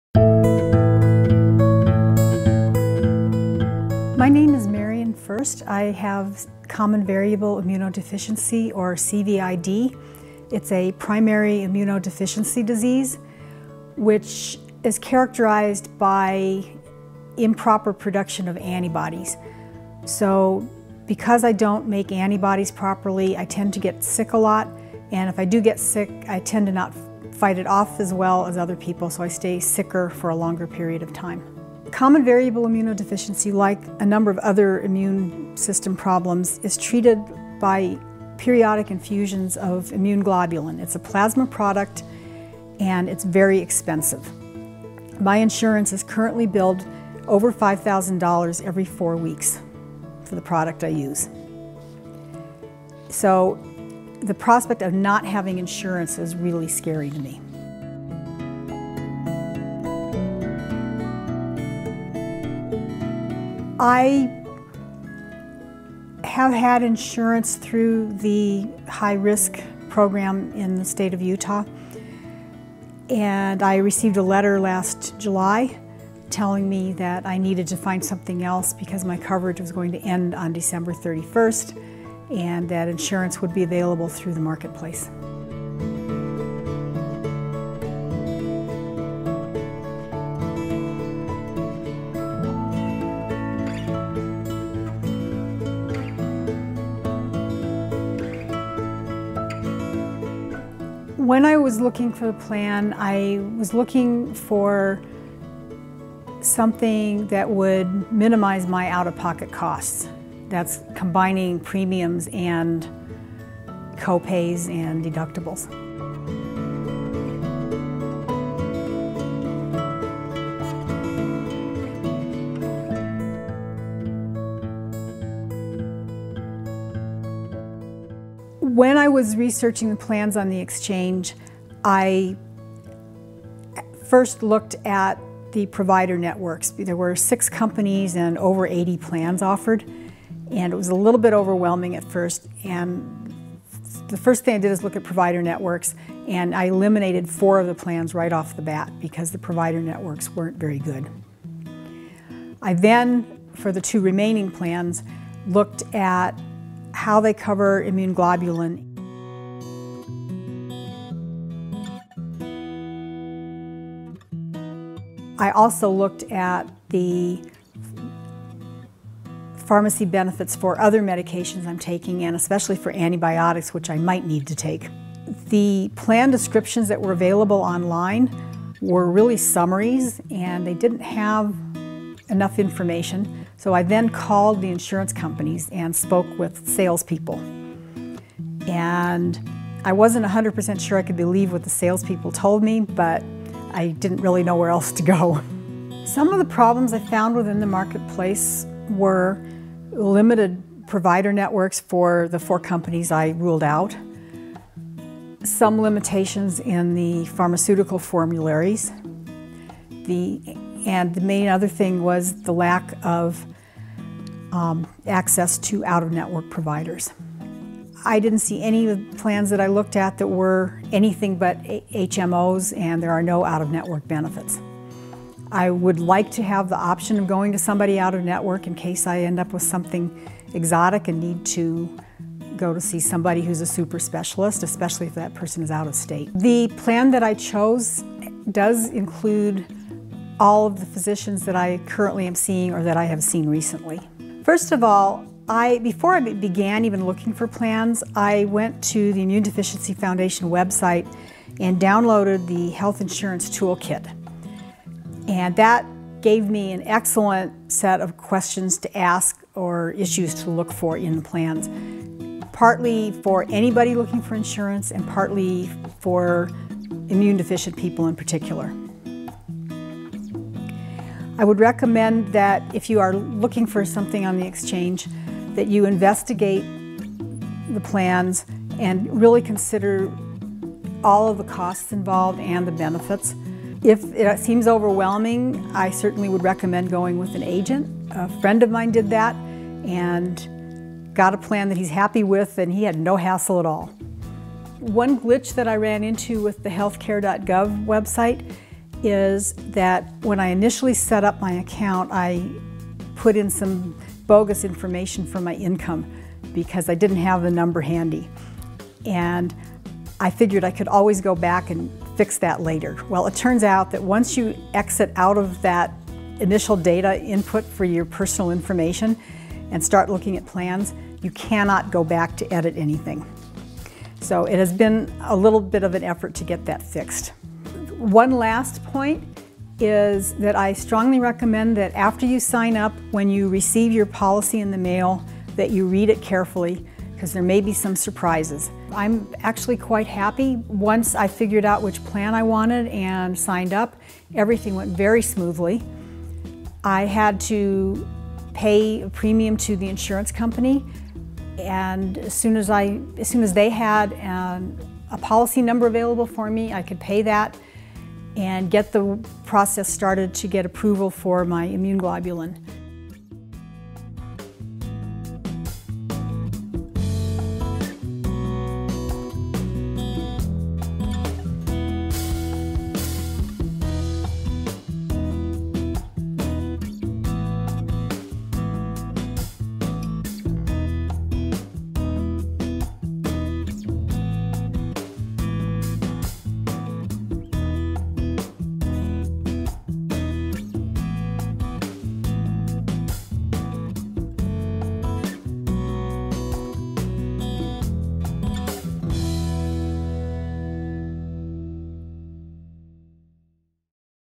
Shopping in the Health Insurance Marketplaces
Marian, a patient with Common Variable Immune Deficiency, details her experience shopping for a health plan in the health insurance marketplace. She gives tips for patients with primary immunodeficiency disease to consider when buying a new health insurance plan.